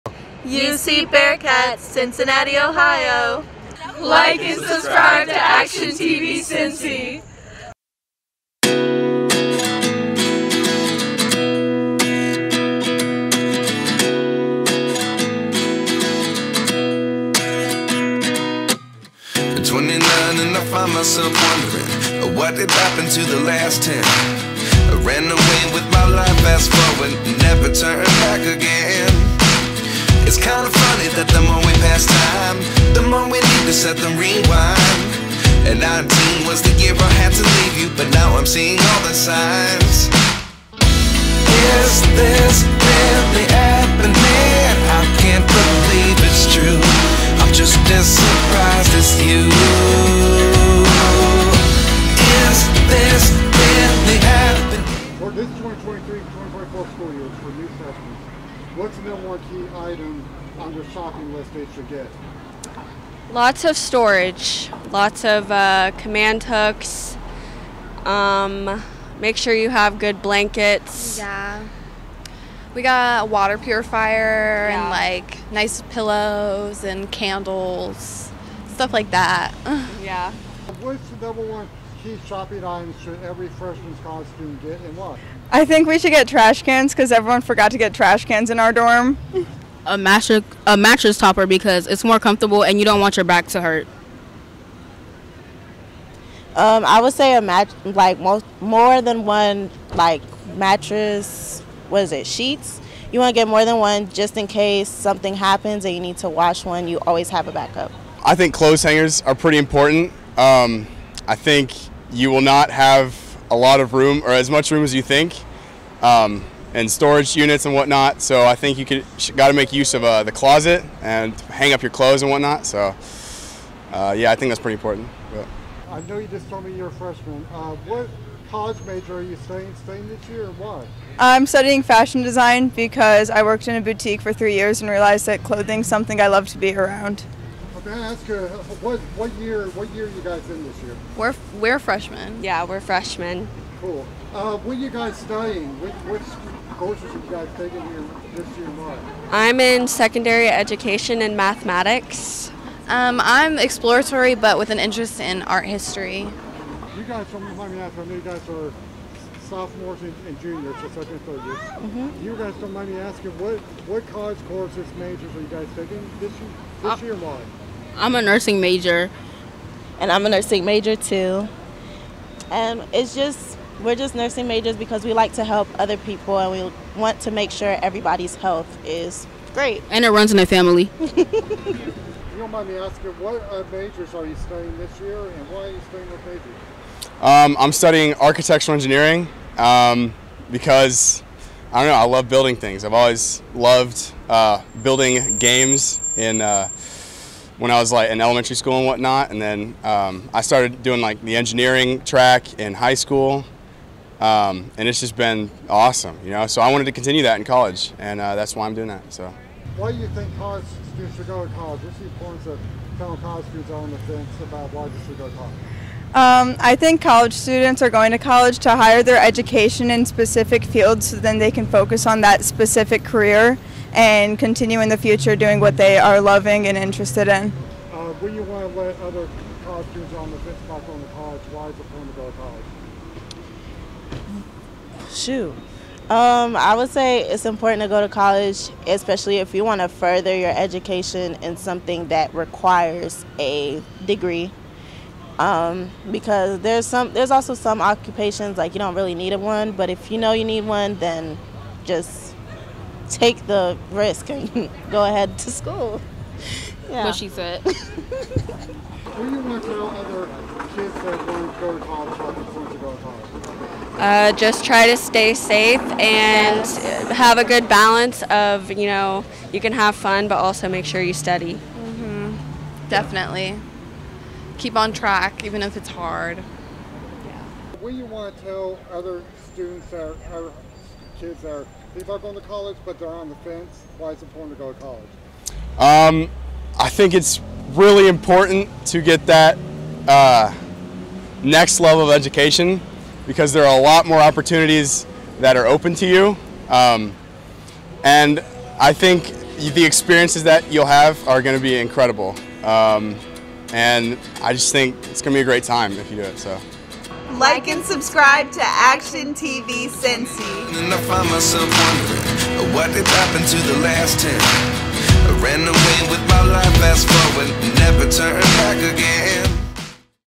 UC Cincinnati What key shopping item should a freshman get?
College is starting up after Summer break. Students need those key
shopping items for the school year? We ask UC students what key
shopping item students need? Leave in the comments if your in school still